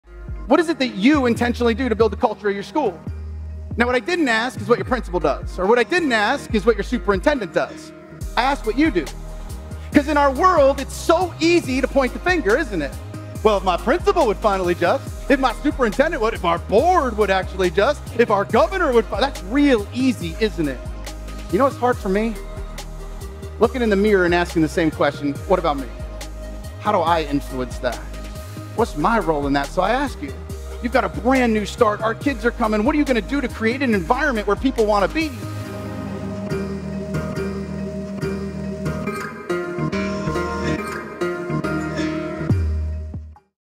What Do You Do to Build Culture?
Pointing the finger is easy. It takes zero courage. Looking in the mirror and reflecting on our own influence on a situation, at least for me, is often much more of a challenge. So, what is it that you intentionally do to create an inclusive culture where people want to be? What works for you?